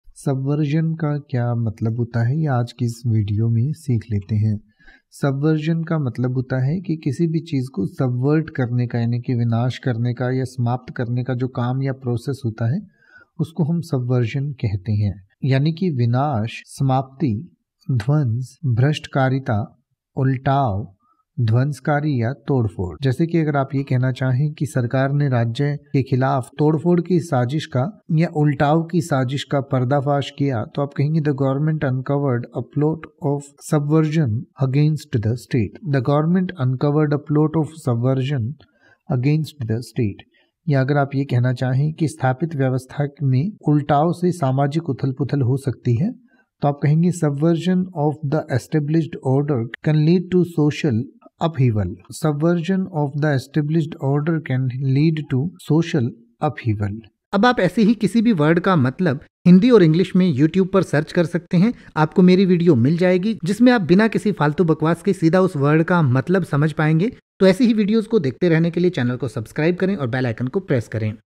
Subversion meaning in Hindi | Subversion ka matlab kya hota hai | English to hindi
इस वीडियो में आप Subversion का हिंदी में मतलब समझेंगे और साथ में Subversion का pronunciation भी सीखेंगें यानि की आप Subversion के अर्थ के साथ साथ ये भी सीखेंगें की Subversion को कैसे बोले या Subversion को बोलने का सही तरीका क्या है |

तो बस एक मिनट में सीखिए Subversion को|
Let’s learn Hindi Meaning of Subversion in detail.

Subversion का हिंदी में अर्थ होता है
विनाश, समाप्ति, ध्वंस, भ्रष्टकारिता, उलटाव, ध्वंसकारी, तोड़फोड़

The government uncovered a plot of subversion against the state.
सरकार ने राज्य के ख़िलाफ़ तोड़फोड़ की साजिश का पर्दाफाश किया।
Subversion of the established order can lead to social upheaval.
स्थापित व्यवस्था में उलटाव से सामाजिक उथल-पुथल हो सकती है।

Subversion को इन उदाहरणों से और अच्छे से समझ सकते हैं